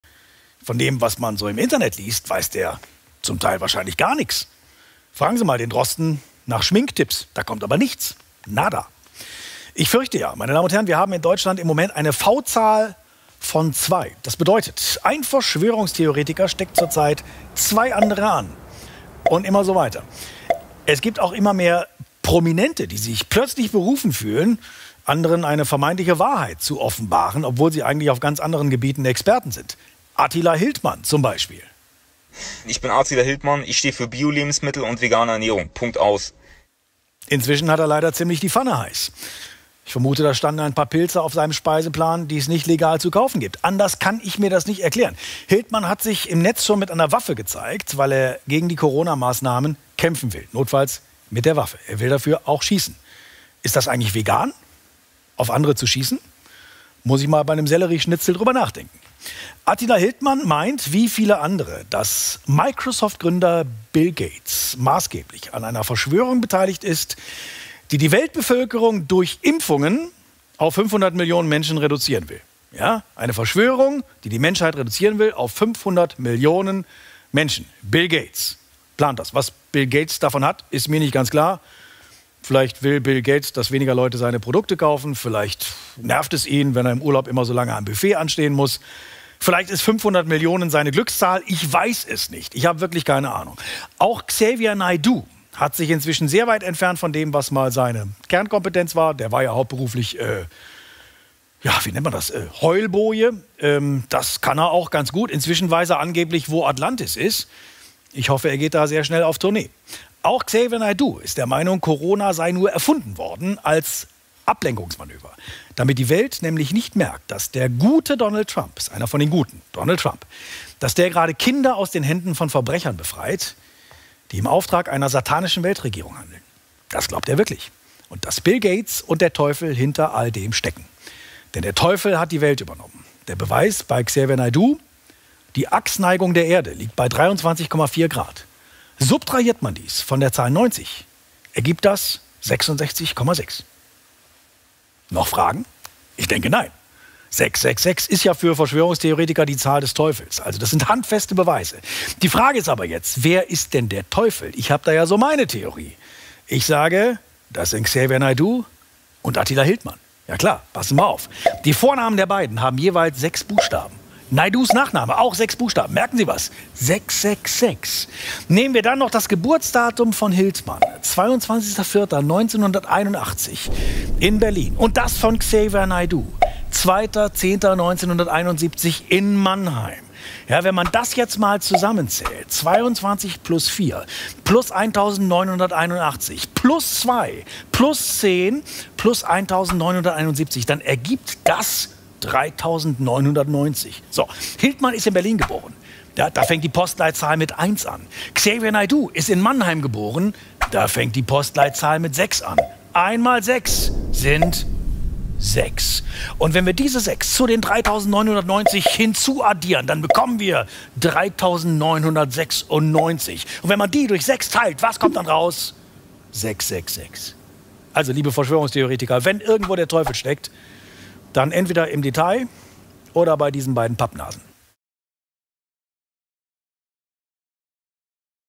NDR redet über | Attila Hildmann| Xavier Naidoo | Corona und Verschwörungstheorien!
Diese Video dient zur Information und nicht zur Panik macherei oder eine oder mehrere Personen schlecht darzustellen!